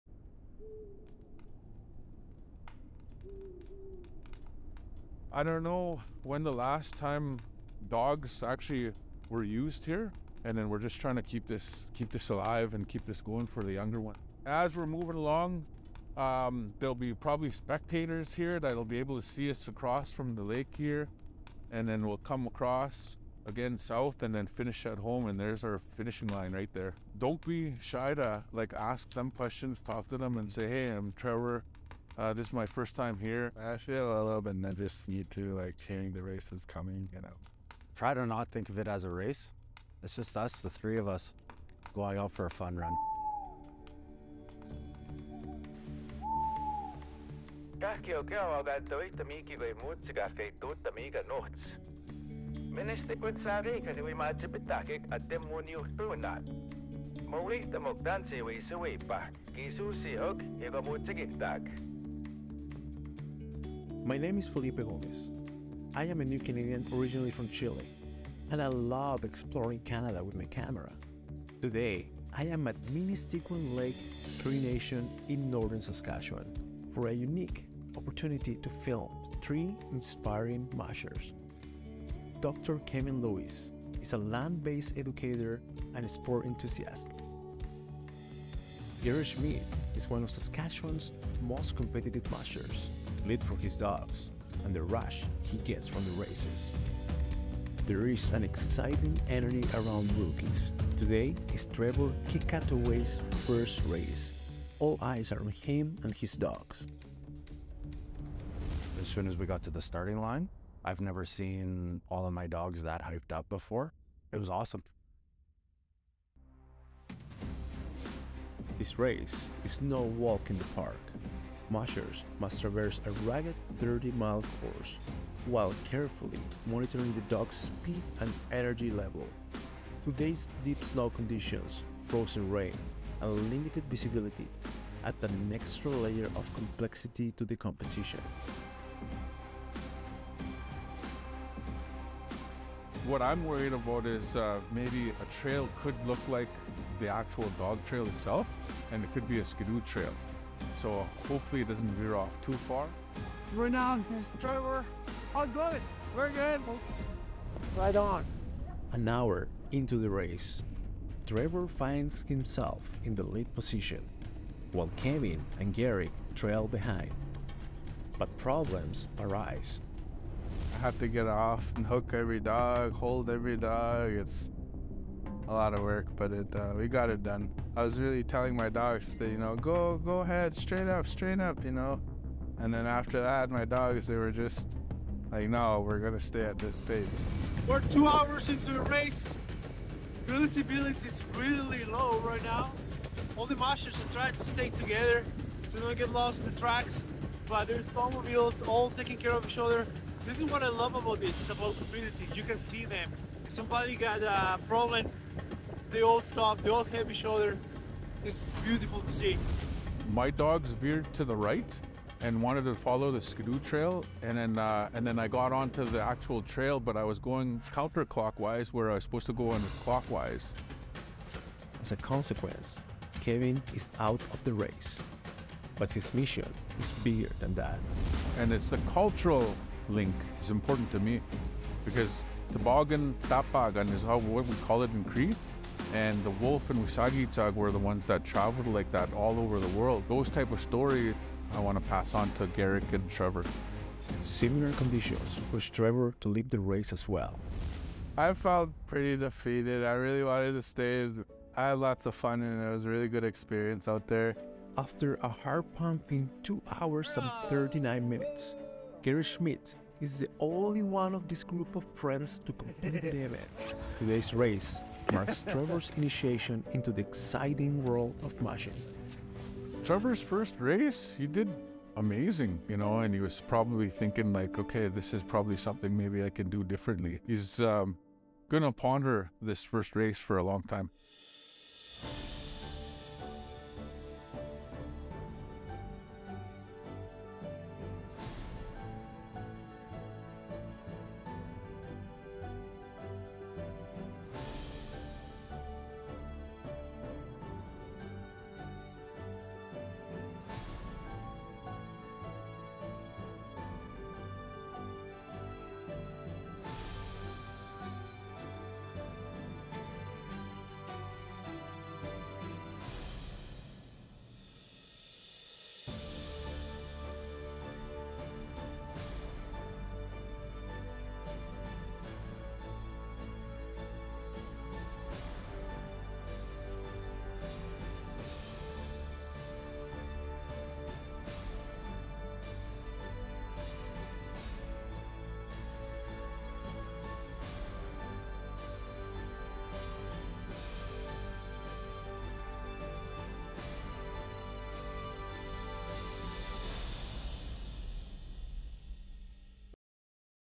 Young dog musher takes part in his 1st race with the help of elder
In this video for the CBC Creator Network, Explorer and Filmmaker Felipe Gomez, a new Canadian originally from Chile, travels to Northern Saskatchewan to meet with young mushers Garrick Schmidt, 19, and Trevor Kequahtooway, 30, from White Bear First Nation.

Garrick and Trevor are becoming dog mushers, training with and learning from head musher and University of Saskatchewan professor Kevin Lewis.

For more information about how you can create video content for CBC, go to cbc.ca/CreatorNetworkSK